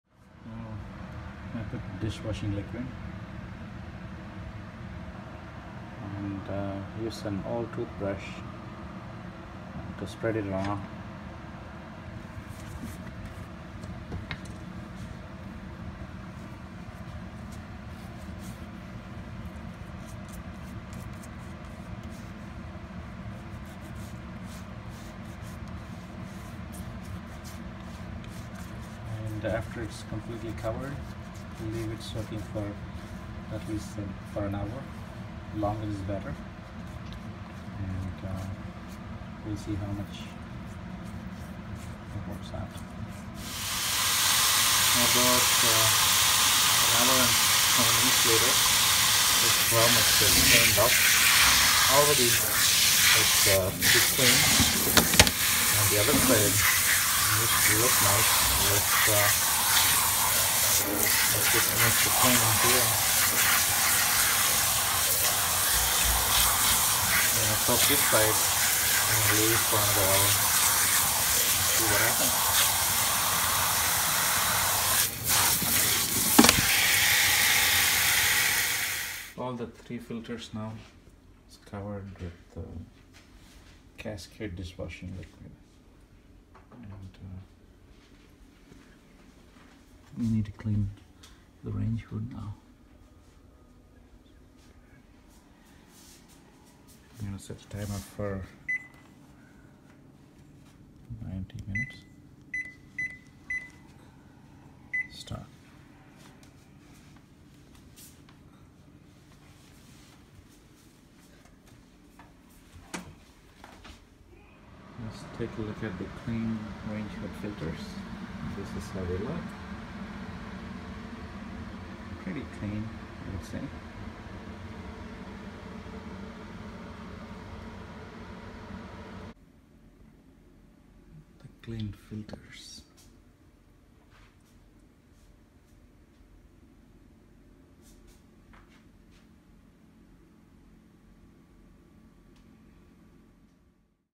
Cleaning Greasy Range Hood Filters With Cascade Dish Washing Liquid
How to clean greasy range hood filters using Cascade dish washing liquid.